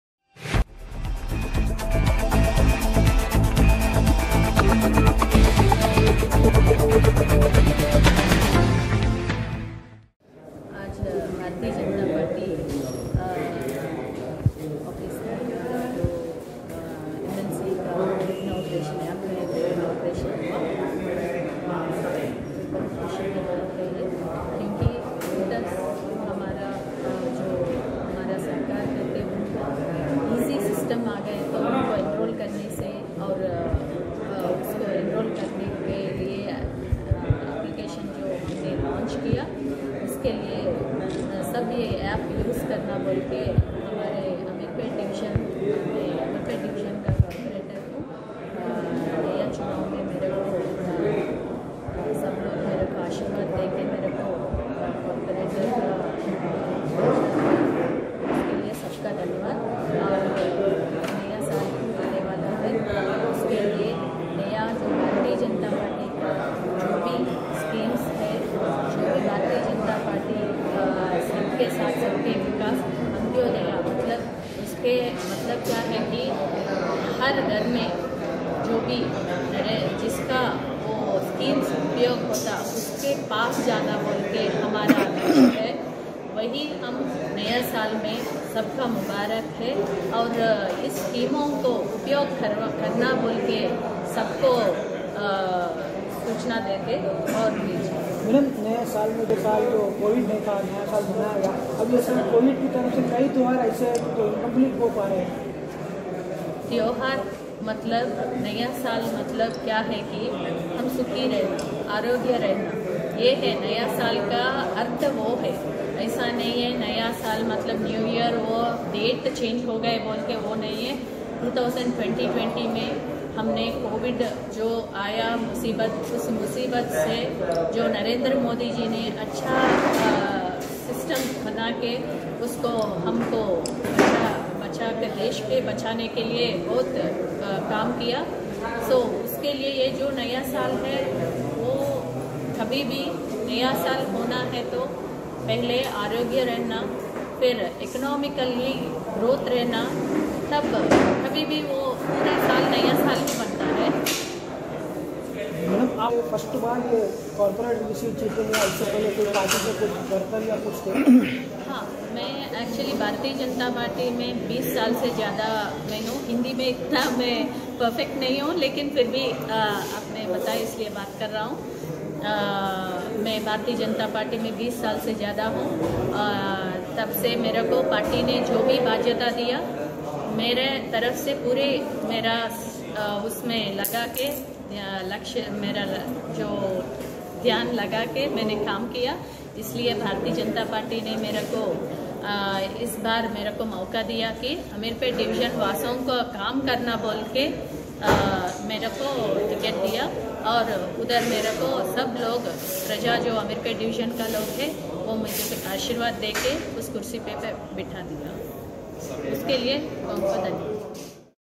Aaj..b.j.p.head office nampelly ma ameerpet division b.j.p.party corporter sarala....
Aaj..b.j.p.head office nampelly ma ameerpet division b.j.p.party corporter sarala.madam na media sa bath kwertha hova new year ki Mubarak bath pesh ki